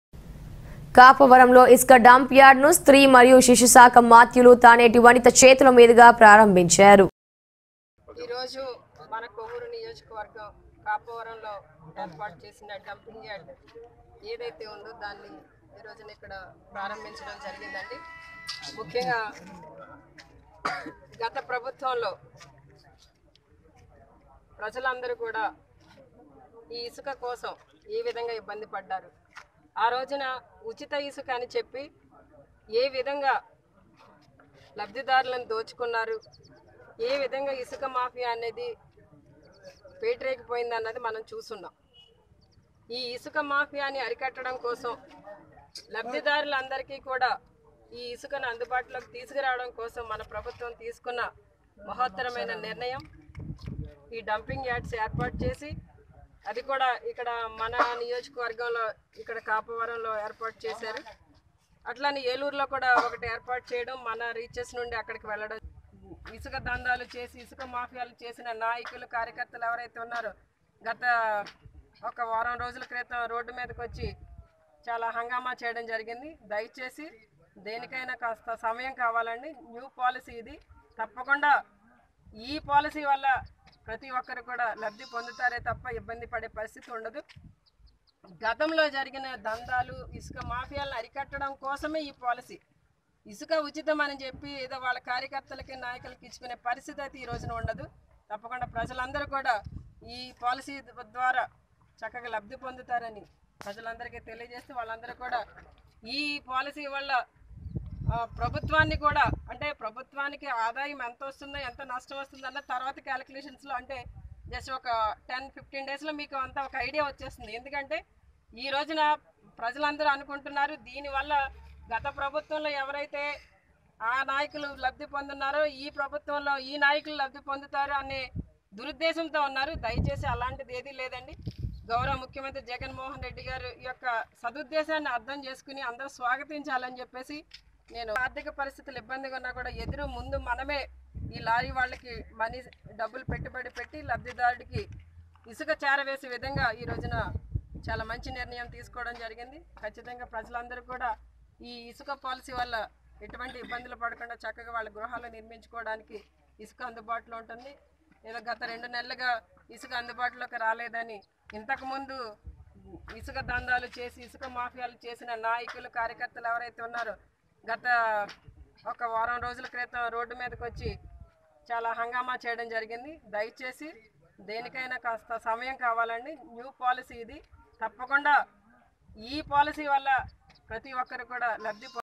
కాపవరంలో ఇసుక డంప్ యార్డ్ ను స్త్రీ మరియు శిశు శాఖ మాత్యులు తానేటి వనిత ప్రారంభించారు
కాపవరం లో ఇసుక  డంప్ యార్డ్ ను  స్త్రీ మరియు శిశు శాఖ మాత్యులు  తానేటి వనిత చేతుల మీదుగా ప్రారంభించారు.